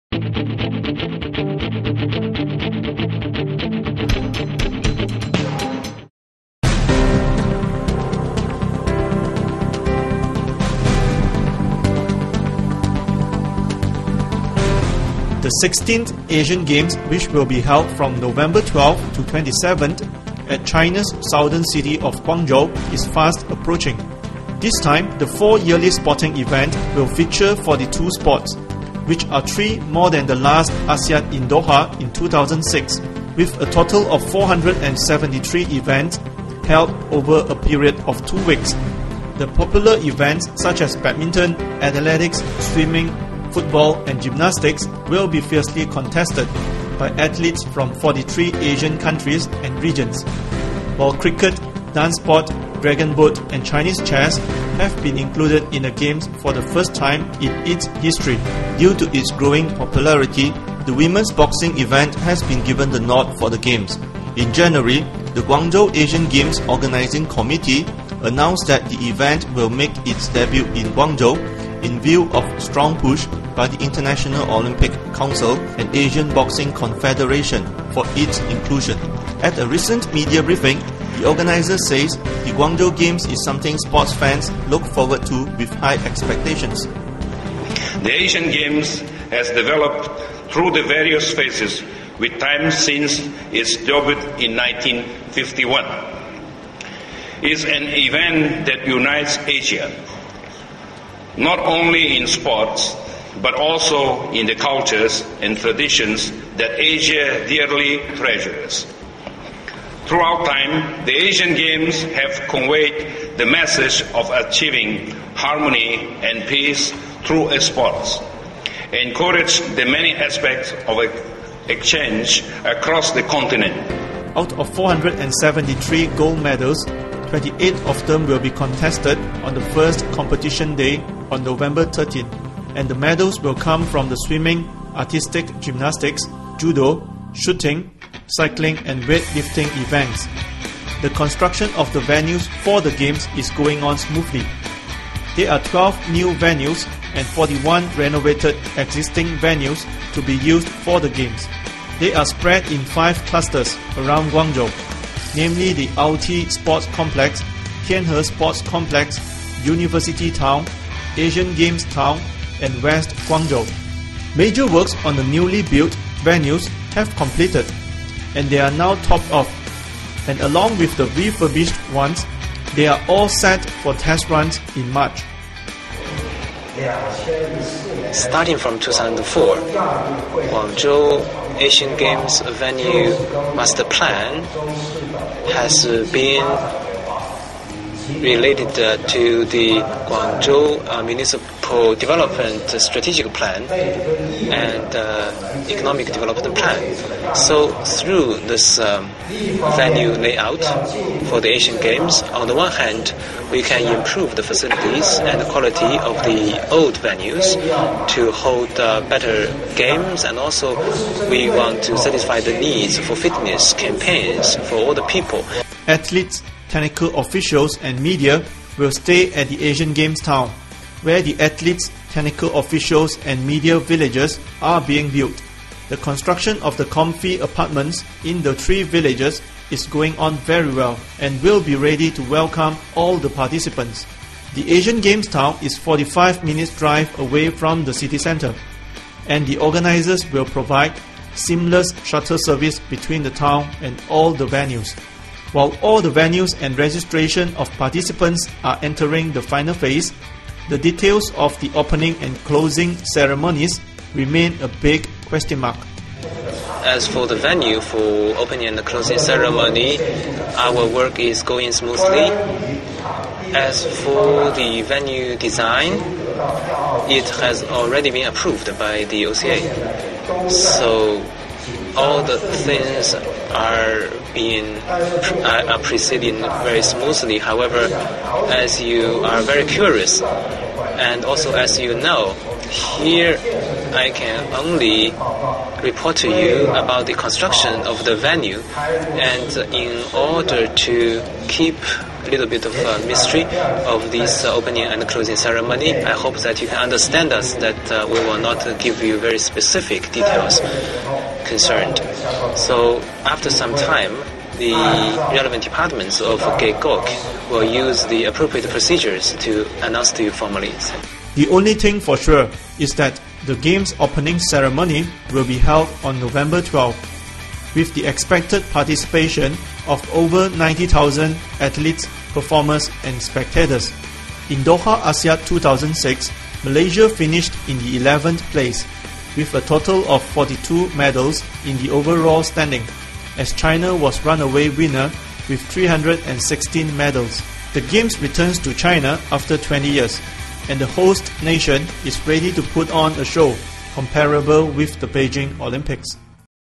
Guangzhou Asiad: We are ready!
The 16th Asian Games will be held at Guangzhou in China from Nov 12-27. The organisers are ready to put on a show comparable with the Beijing Olympics. Chow How Ban reports from Guangzhou.